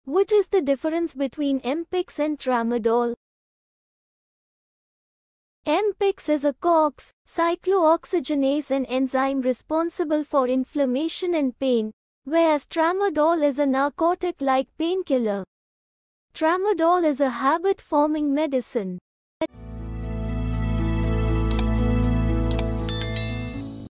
What is the difference between M Pix and tramadol?
M Pix is a COX (cyclooxygenase -an enzyme responsible for inflammation and pain) whereas Tramadol is a narcotic-like painkiller. Tramadol is a habit forming medicine.